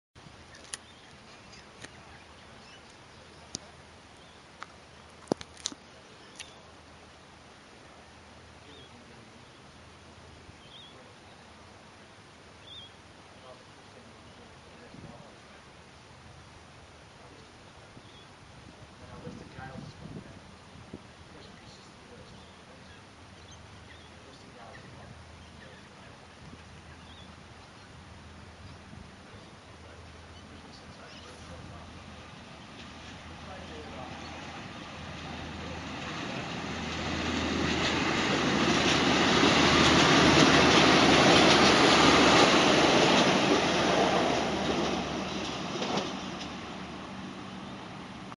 Train Travelling Through Royal Adelaide Golf Club
How's this for something different? A passenger train line running through the middle of a golf course. The train track is an integral part of the course and any ball resting on the track is to be played as it lies or relief can be taken with a penalty. The train track does not really interfere with normal play and only the very errant shot would be affected. A great course overall.